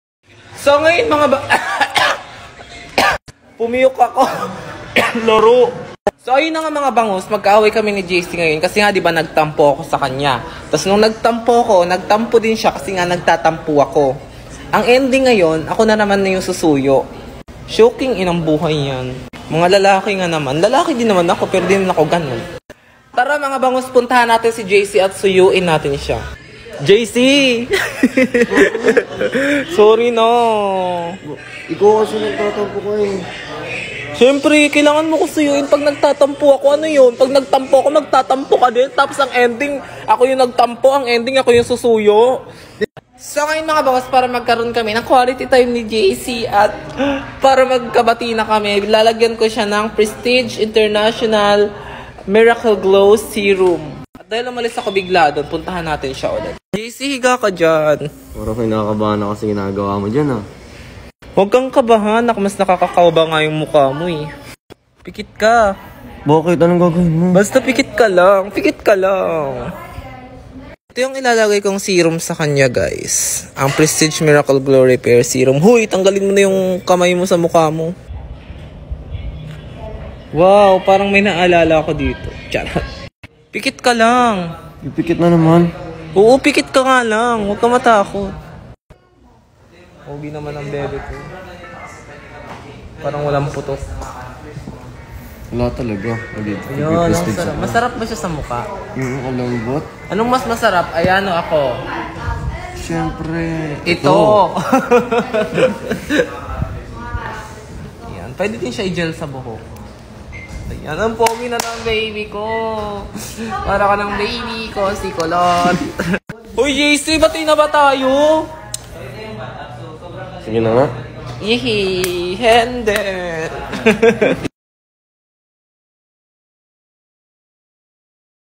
inaway ni Kuya jc si ate riri pero ang ending si ate riri ang nag suyo
jcri jcriupdate inaway ni Kuya jc si ate riri pero ang ending si ate riri ang nag suyo haha😄

follow-my intagram name-jayson_sartorio and jayson1234
follow-my tiktok name-jester dave sartorio